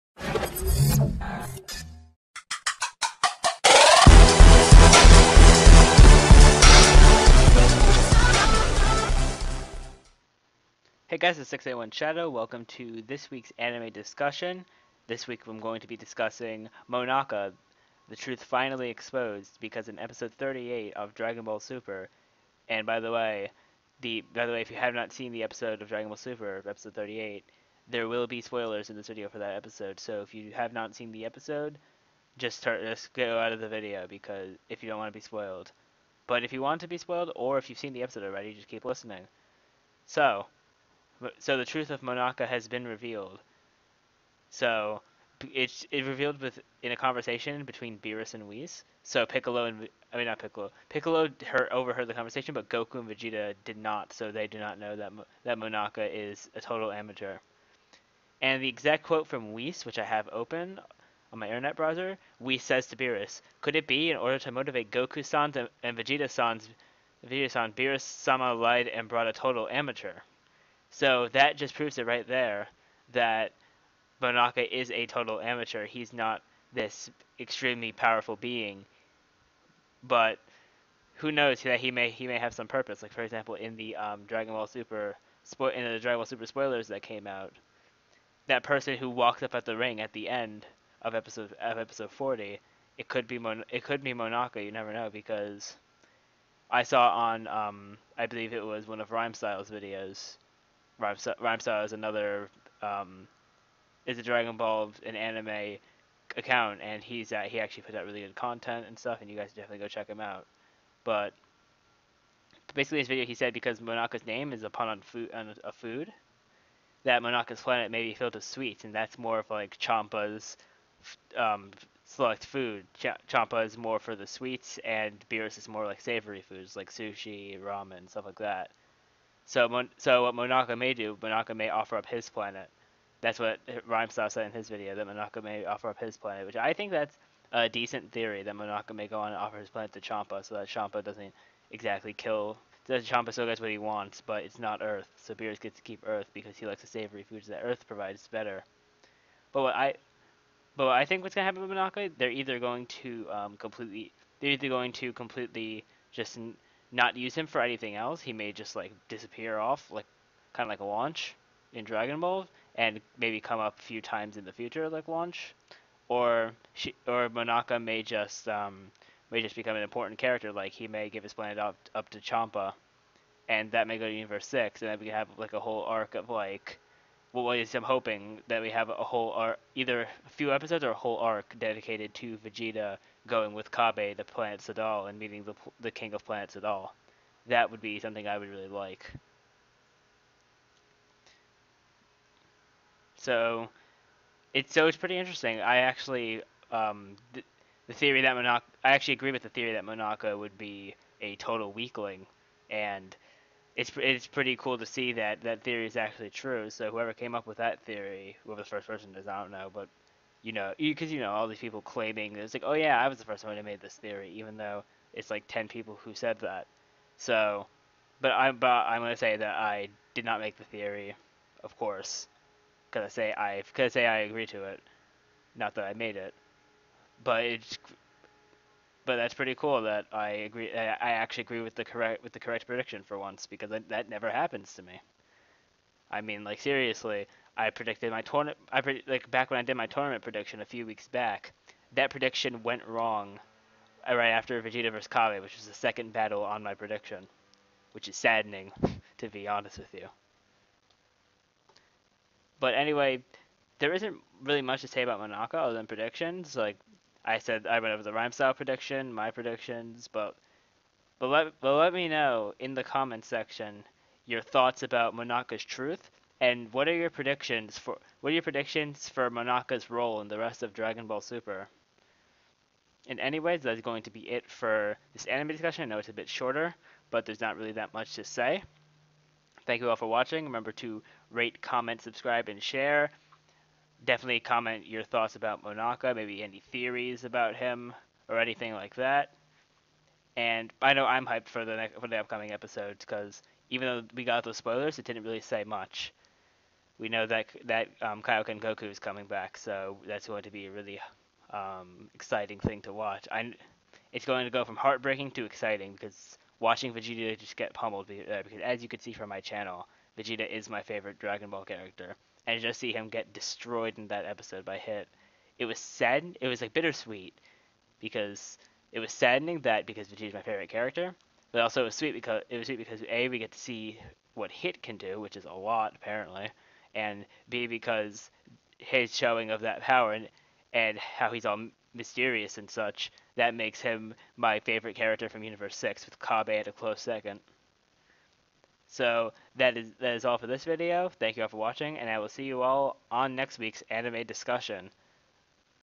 Dragon Ball Super Discussion - Monaka's Truth Revealed
Here is another Anime Discussion Video! In this Discussion, I discuss my thoughts about the truth of Monaka! I also discuss my predictions for Monaka in the rest of the series!

My Links!
Twitch.tv/681shadow
Twitter.com/681shadow